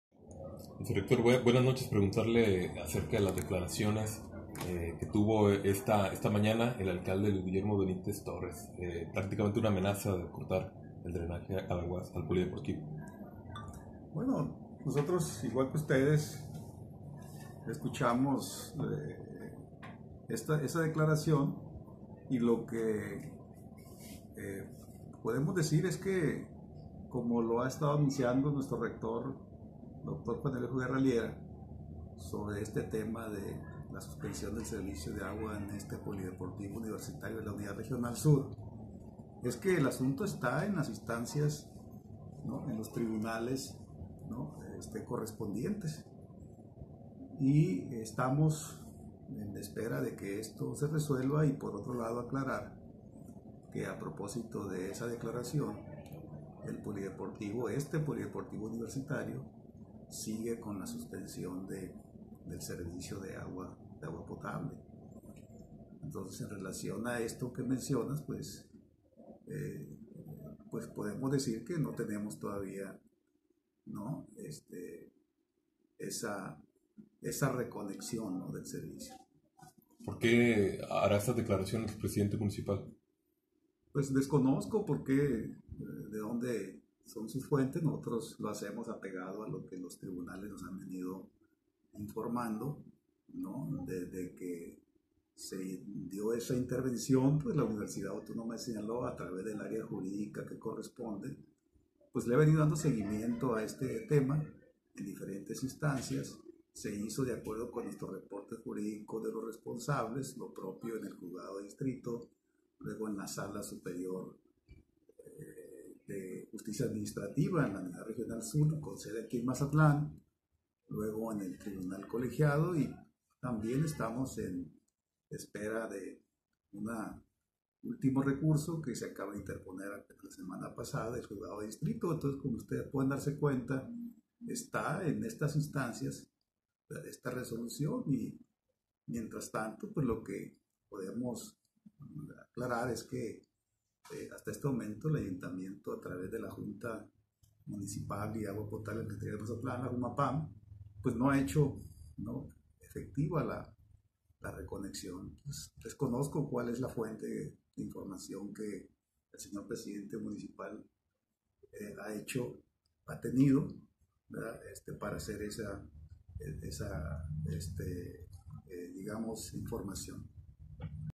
SIGUEN SIN ACATAR RECONECCIÓN DEL SERVICIO DE AGUA POTABLE AL POLIDEPORTIVO: VICERRECTOR
El tema sigue en las instancias jurídicas aclara Miguel Ángel Díaz Quinteros.
La Jumapam no ha realizado la reconección del servicio de agua potable y drenaje al Polideportivo de la UAS en Mazatlán aclara el vicerrector de Unidad Regional Sur, Miguel Ángel Díaz Quinteros.
Precisó que tal como lo ha anunciado el propio rector, Juan Eulogio Guerra Liera, el tema se esta ventilando en las instancias jurídicas y los tribunales correspondientes, estando a la espera de que se resuelva.
Lo anterior, fue precisado, a raíz de las declaraciones del alcalde Luis Guillermo Benítez Torres en el sentido de que reconecto el servicio de agua potable a raíz de una orden judicial, la cual afirmó se acató por una cuestión de salud pública, aunque están analizando restringir el drenaje como una medida de presión.